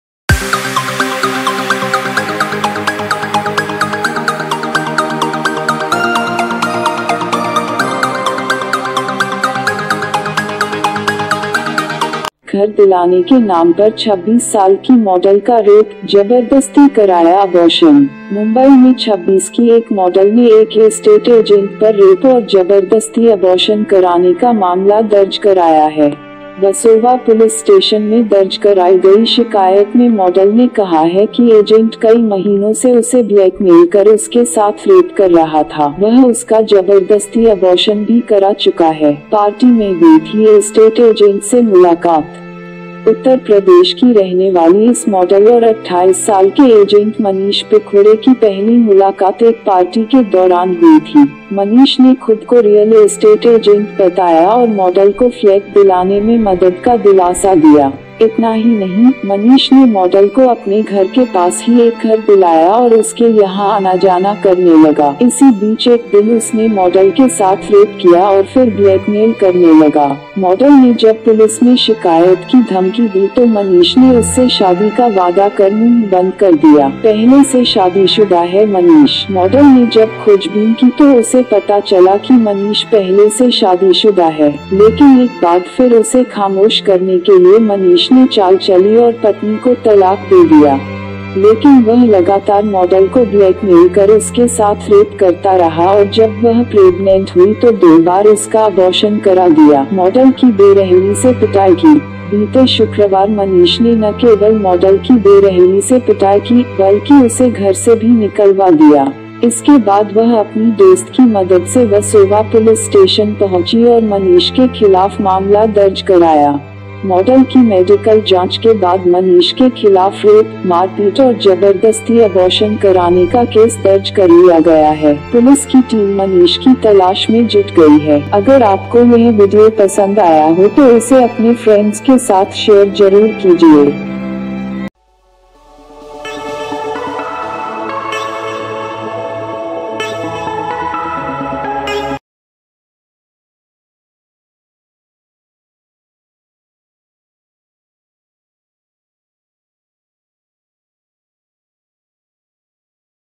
घर दिलाने के नाम पर 26 साल की मॉडल का रेप, जबरदस्ती कराया अबॉर्शन
घर दिलाने के नाम पर 26 साल की मॉडल का रेप, जबरदस्ती कराया
 Real Estate Agent Raped 26 Years Old Model In Mumbai
Home rape of 26 year old model on the business name, forcefully made abortion

A model of 26 in Mumbai lodged a case of rape to an estate agent and forced abortion.

Real Estate model agent accused of rape.
Mumbai. A model of 26 in Mumbai lodged a case of rape to an estate agent and forced abortion. Versova model went complaint lodged with the police station said that agents had blackmailed him for several months had been raped. He is also the forced abortion. The party was met by estate agents ...

- The first meeting of Uttar Pradesh resident agent of this model and 28-year-old Manish Pikvde took place during a party. Manish said real estate agent himself and model console help the apartment business.
- Not only that, Manish said model started just made a visit to his home and then come here. Meanwhile, the rape of a day she models and then began to blackmail.
- The model has since threatened to complain to the police, Manish had silenced the promise to marry her.
Already married Manish
- model of the research he learned that Manish is already married. But once again went Manish's move to silence and his wife divorced.
- but he can blackmail the constant model been raped and when she was pregnant made her abortion twice.